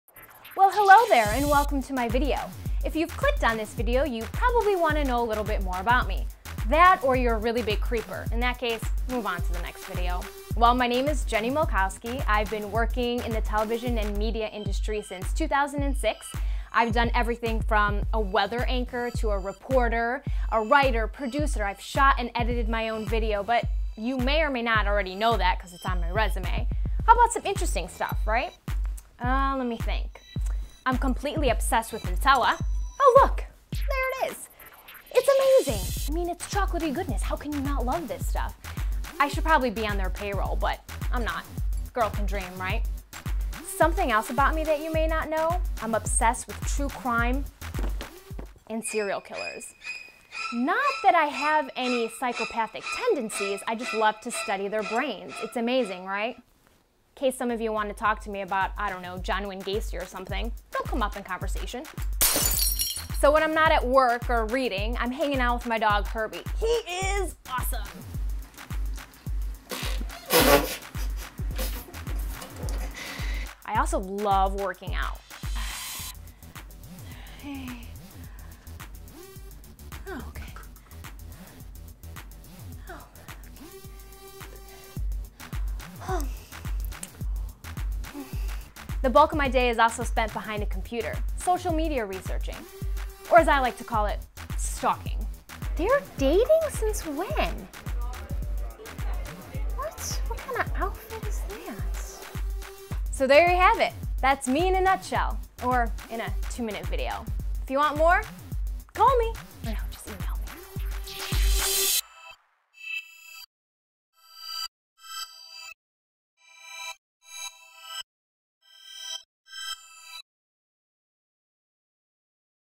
A Little Bit About Jenny Milkowski
Some stuff you probably don't know, or don't want to know. about Jenny Milk.

(no teleprompter. totally improv)

Oh, and here's some stuff you SHOULD know:

I'm Jenny Milkowski but people call me Jenny Milk and I've been working in the television and media industry since 2006.

I have extensive experience with nearly every position-- including on-air hosting and reporting (both live and taped and with and without teleprompter) weather anchoring, voice-over, producing, writing, shooting and editing.

I ran a successful humor blog about relationships for two years (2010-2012)---and hit 1.43 million page views, in one month alone.

I'm also a graduate of the Improvisation Program from The Second City Chicago and am a student at Acting Studio Chicago.